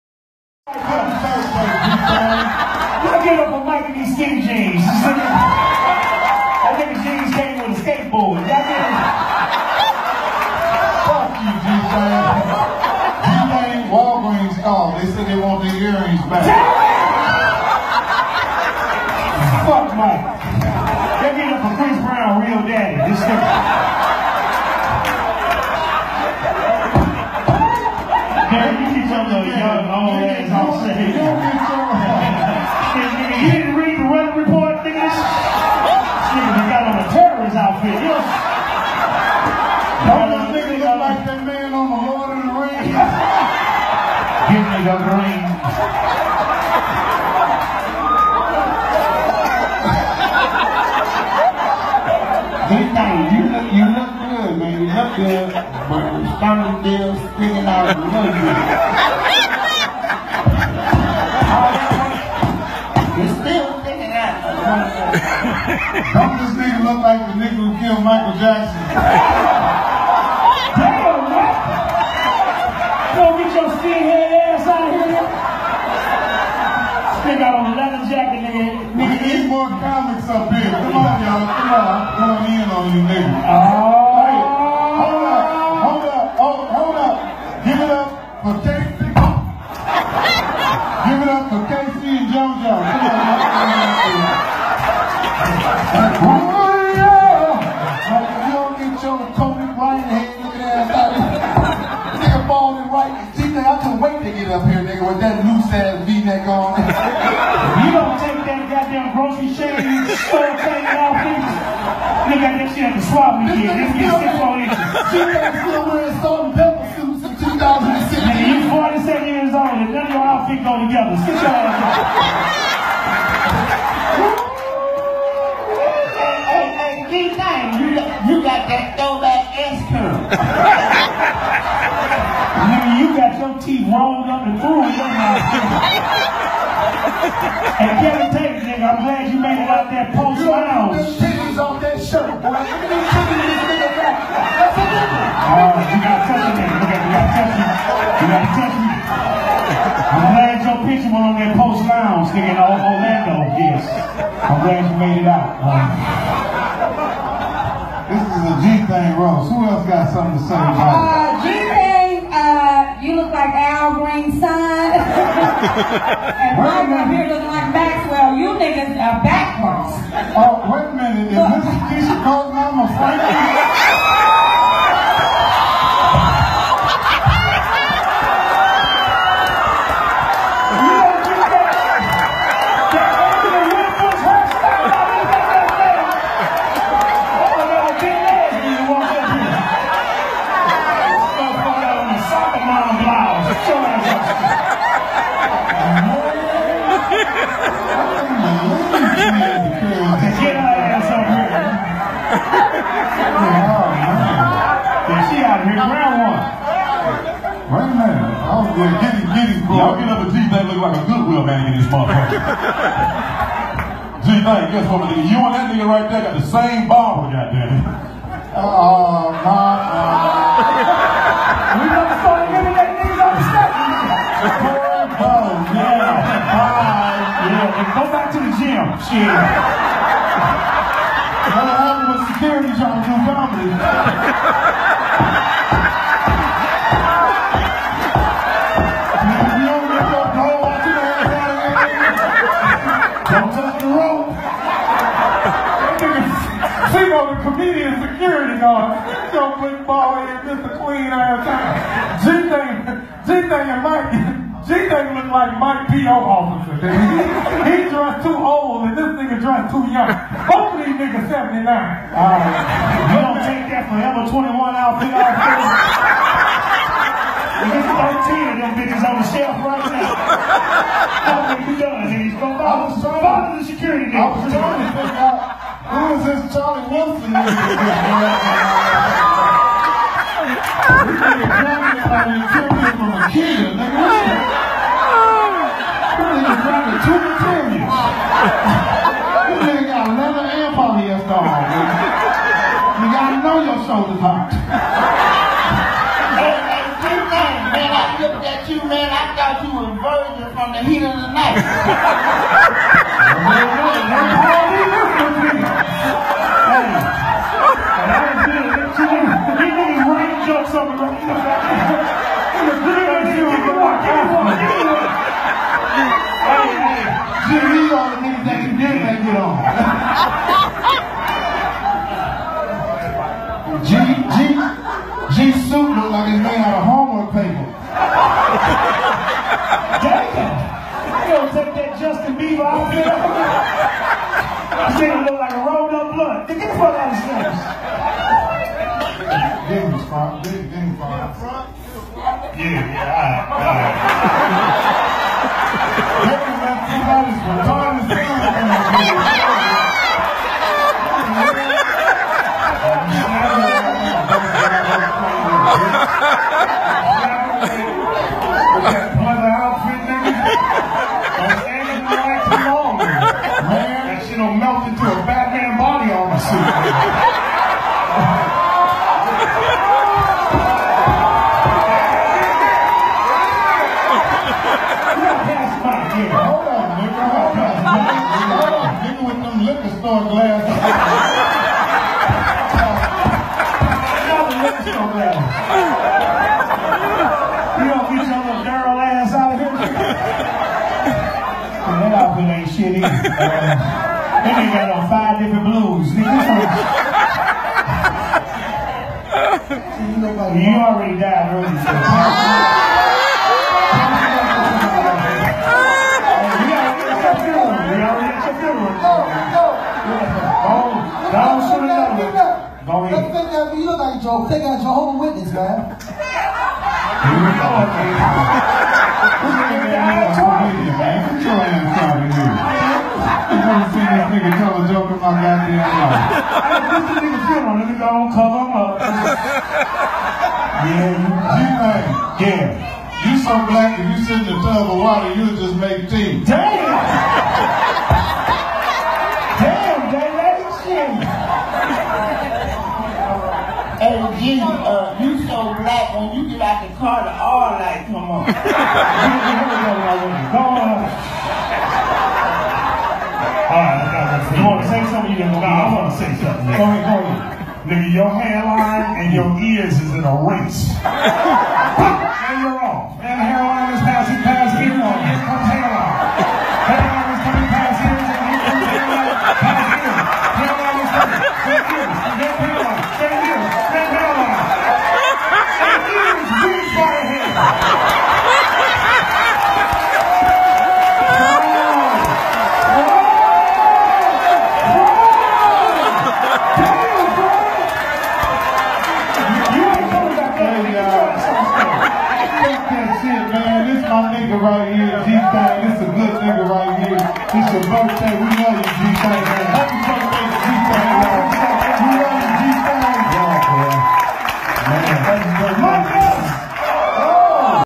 Gary @2Gthang Johnson bday
Mannnn if this ain't some of the funniest shit I've seen in a lonnnng time, if yall don't laugh yall asses off... Gary Gthang Johnson bday was off da chain
SHOW MORE